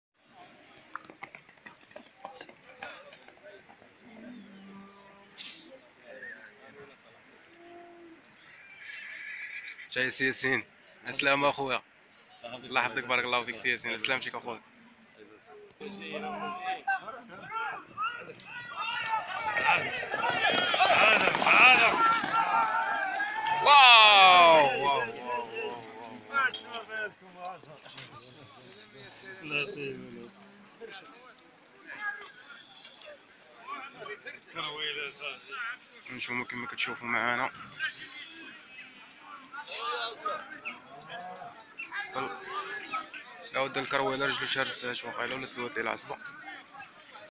اصطدام خطير بين الخيل في سوق السبت بسطات
الحمد لله خرجات سالمة